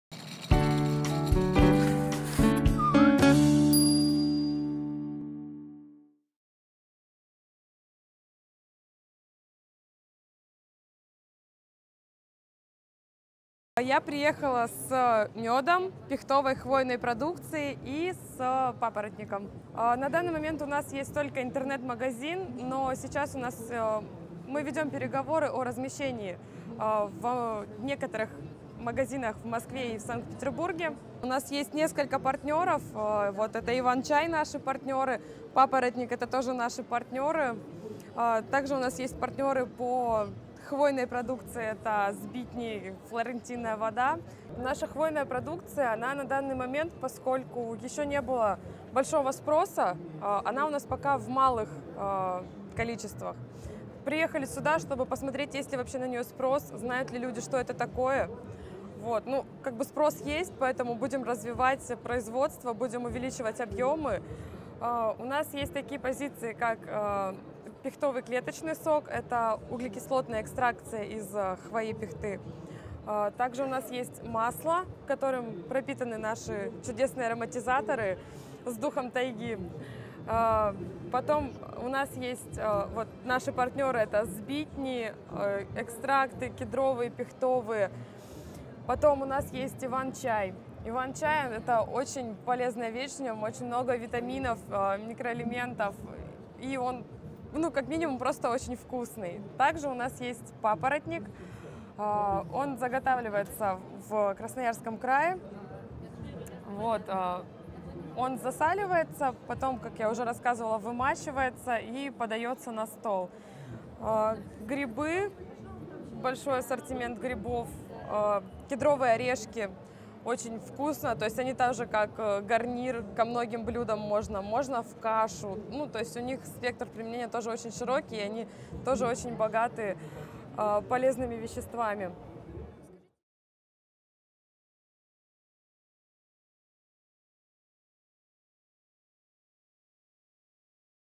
Интервью для retail.ru в рамках проекта “Товар на полку” от "Дух Тайги". Петерфуд 2017
Рудакова Наталья, представитель компании “Дух Тайги” из Красноярского края привезла на Петерфуд преимущественно мед, пихтовую и хвойную продукцию, Иван-чай и довольно необычный продукт на рынке натурального продуктового ритейла — папоротник. Обычно в продаже в засоленном виде. Употребляется в пищу как отдельная закуска или компонент к салатам и пр.блюдам. Папоротник хранится 2 года.
Наталья рассказала нам о выходе с аутентичной продукцией на зарубежный рынок и о личном производстве меда и хвойной продукции.